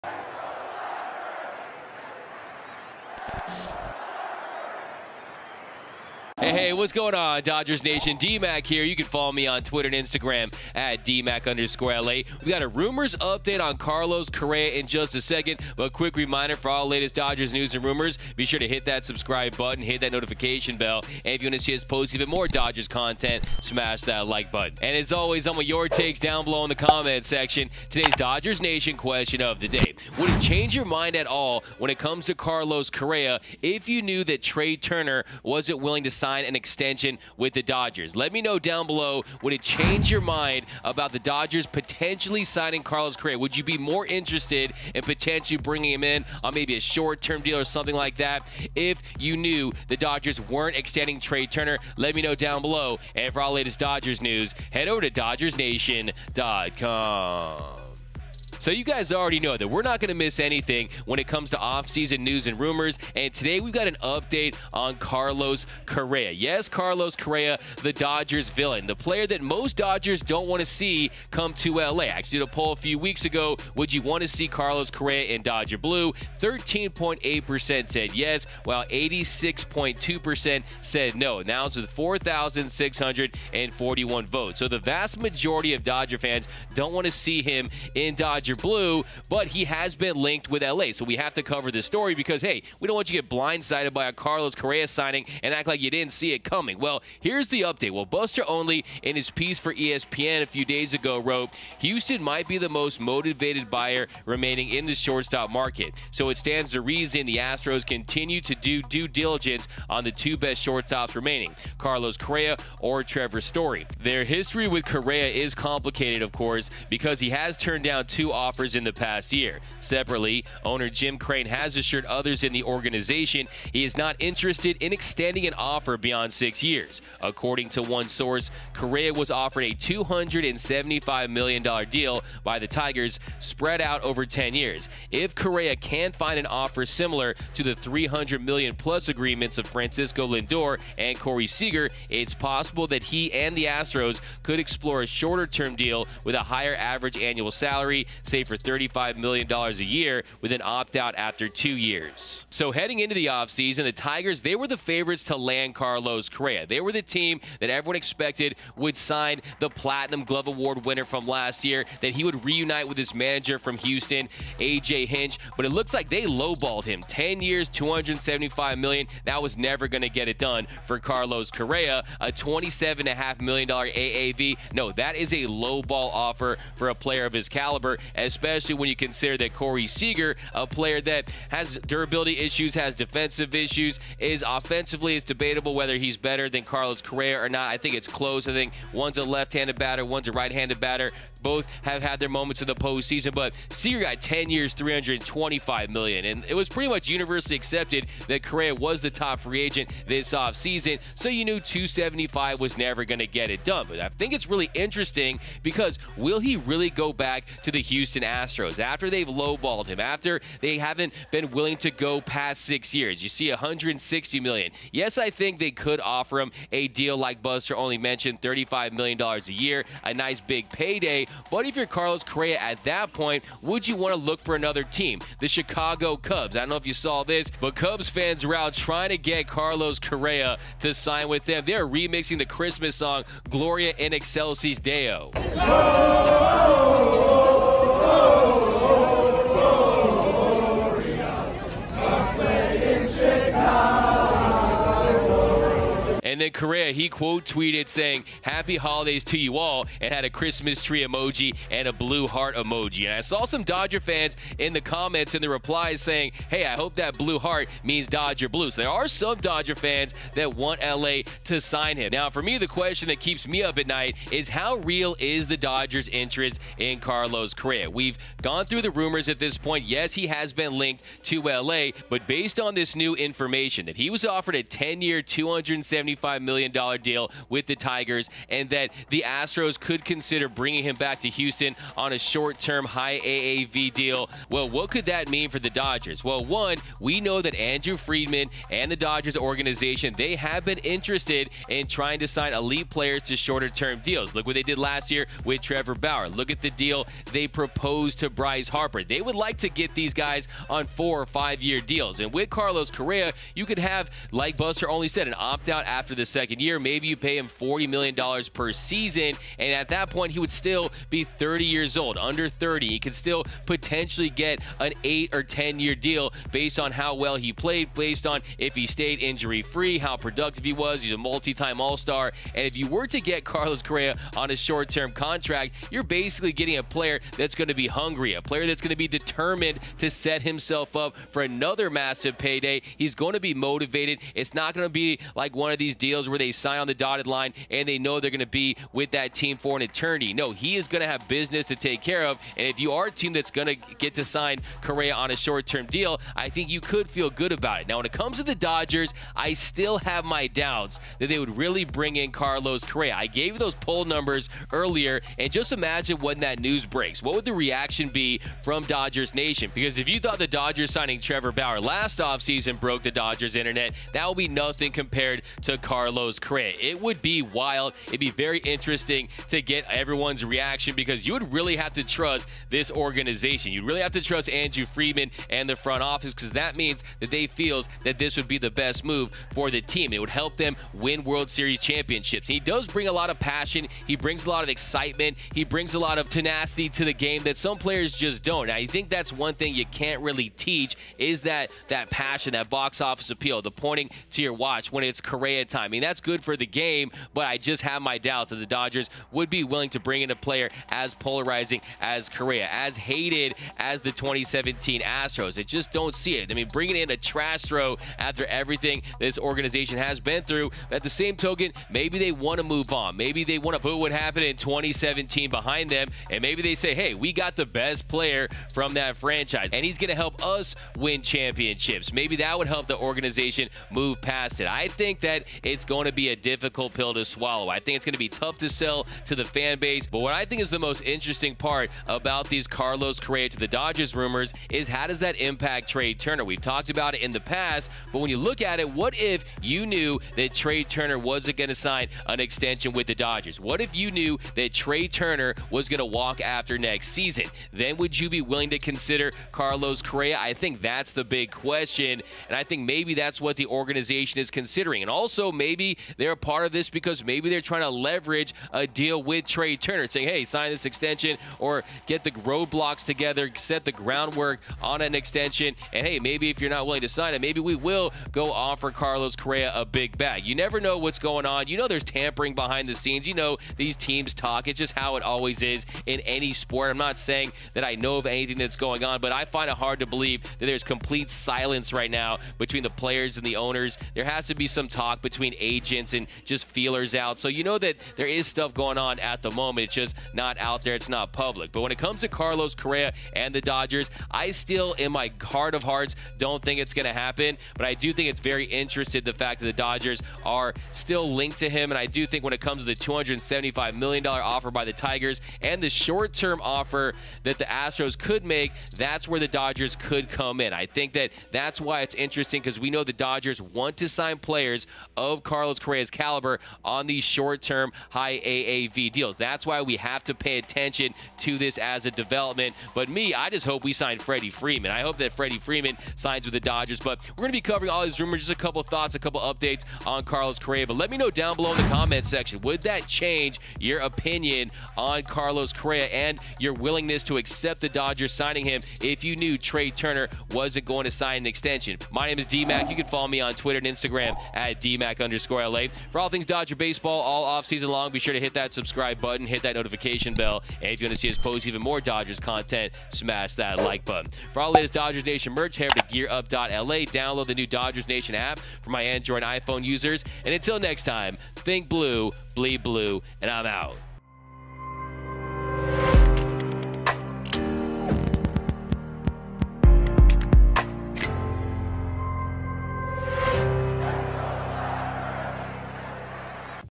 Dodgers Rumors: Carlos Correa Could Sign Short-term Deal, Tigers Big Offer, Is LA Really Interested?
Carlos Correa Could Sign Short-term Deal, Tigers Made Big Offer, Are Dodgers Seriously Interested?

YouTube.com/DodgersNationTV
Instagram.com/officialdodgersnation
Twitter.com/dodgersnation
Facebook.com/dodgersnation